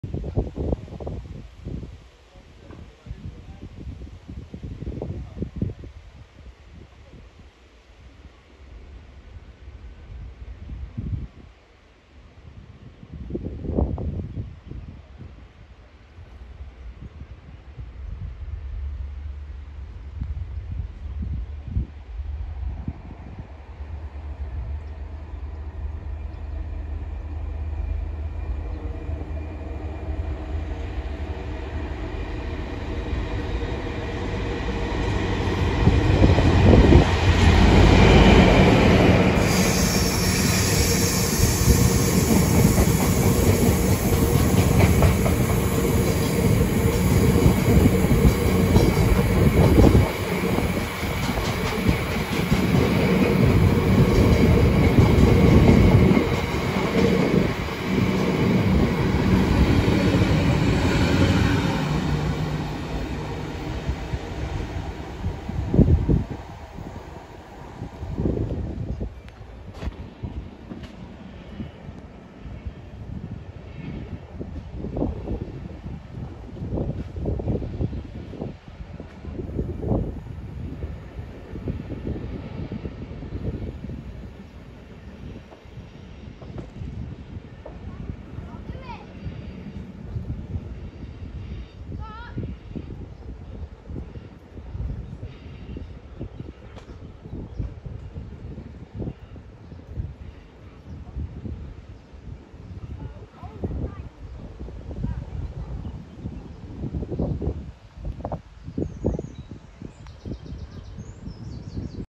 69007 “Richard Trevithick” sand train + 66724 “Drax power station” on tow 13:20 21E Lea Road 5/7/24
This is why we love the sand train! 69007 “Richard Trevithick” 6E85 Middleton Towers Gbrf to Barnby Dun Rockware Glass Gbrf with 15 sand wagons + 66724 “Drax power station” on tow 13:20 running 21 minutes early Lea Road Gainsborough 5/7/24

Last train;

Next train;